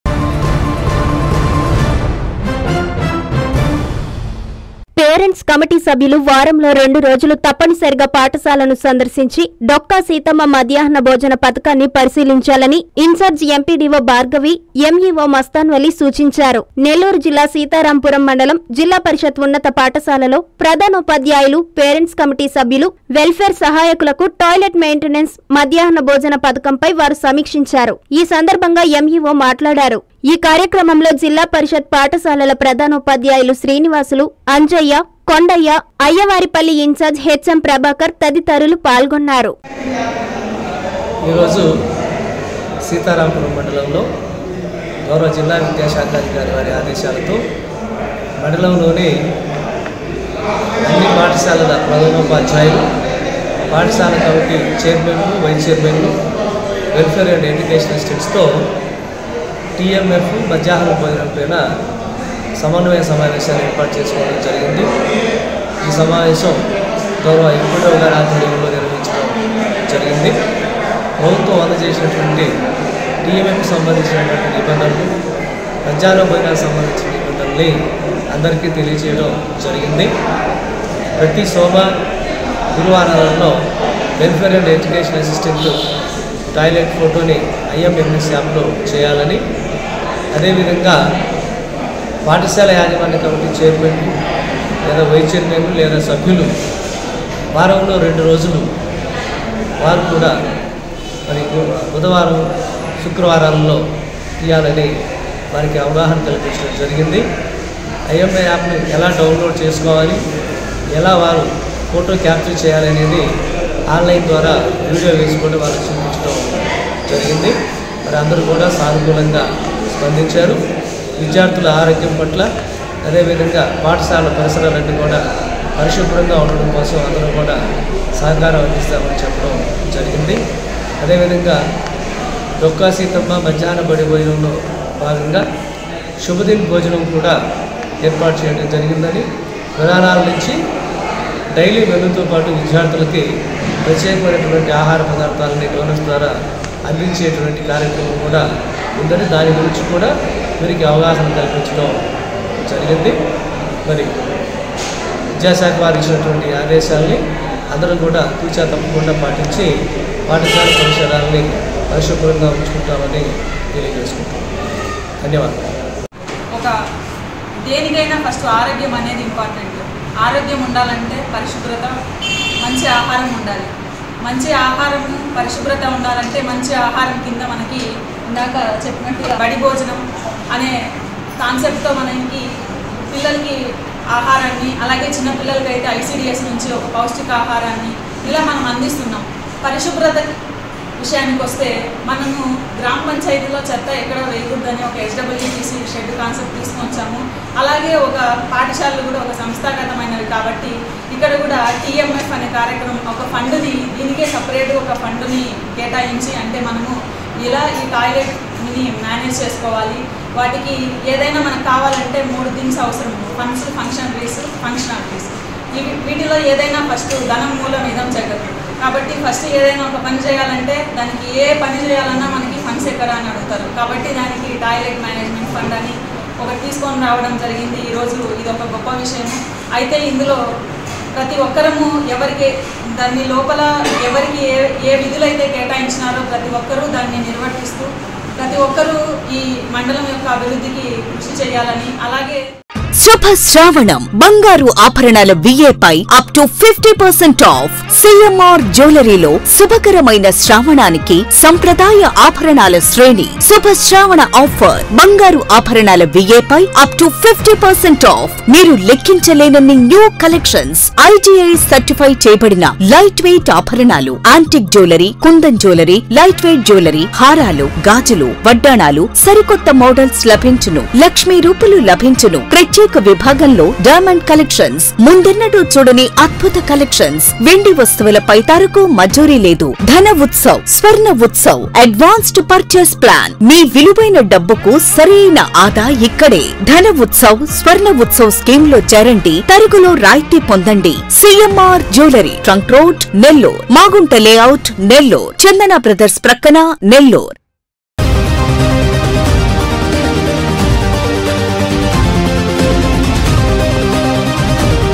వారంలో రెండు రోజులు త‌ప్ప‌ని స‌రిగా...
- డొక్కా సీత‌మ్మ మ‌ధ్యాహ్న భోజ‌న ప‌థ‌కాన్ని ప‌రిశీలించాలి
- ఇన్‌చార్జి ఎంపీడీవో భార్గ‌వి, ఎంఈవో మ‌స్తాన్ వ‌లీ
- సీతారామ‌పురంలో టాయిలెట్ మెయింటైనెన్స్ ,మద్యహ్న భోజన పథకం సమీక్ష